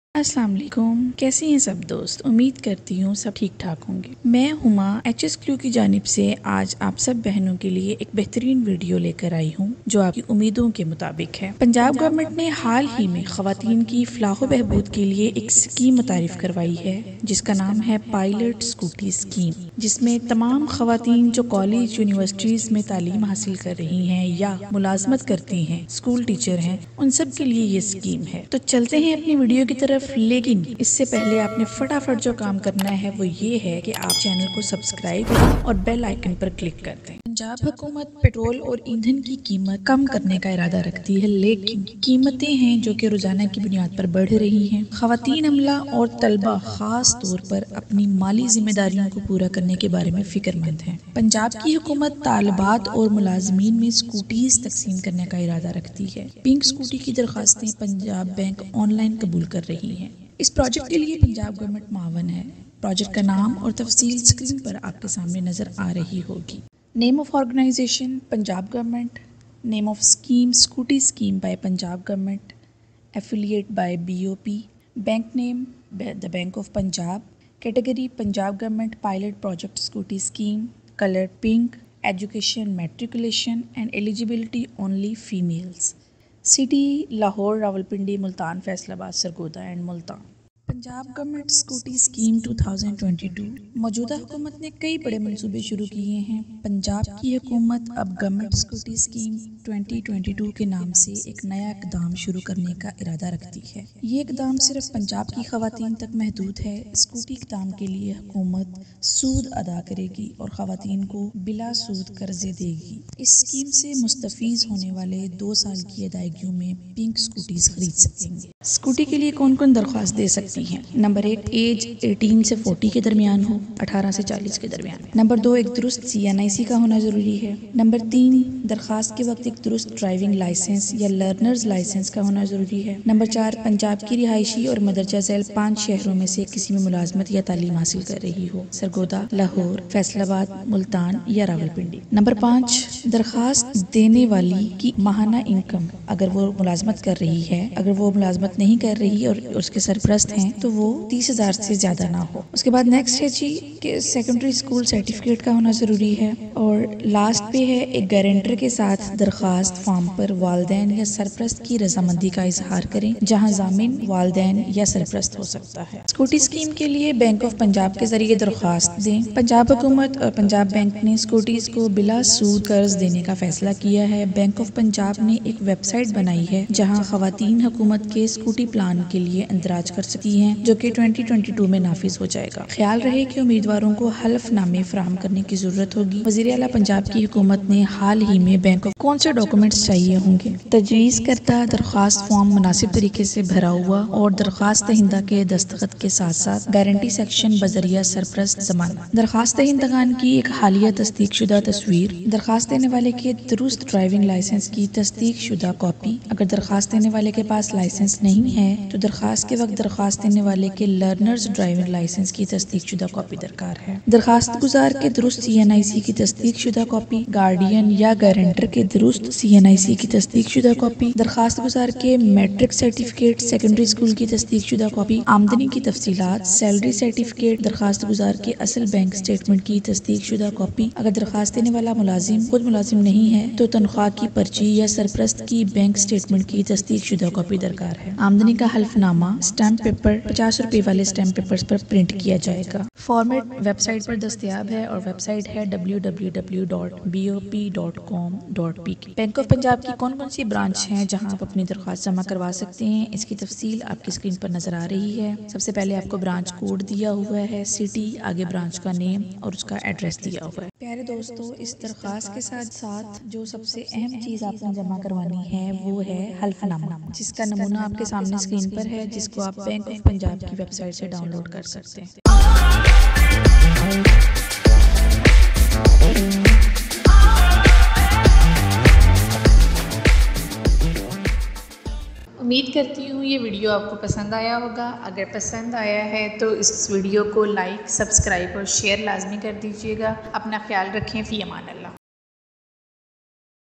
Pink scooty scheme
govt scheme#pilot scooty project
The Punjab government intends to reduce the cost of petrol and fuel. Oil prices are rising on a daily basis. Female staff and students, in particular, are concerned about meeting their financial obligations. Punjab’s government intends to distribute Scotties to female students and employees. Pink Scottie’s applications are being accepted online by the Punjab Bank.
Many major projects have been started by the current government. Punjab’s government now plans to start a new initiative called the government Scooty Scheme 2022. The initiative is limited to Punjabi women. For the Scooty initiative, the government would pay interest and give interest-free loans to women. Beneficiaries will be able to purchase Pink Scotties in two-year payments.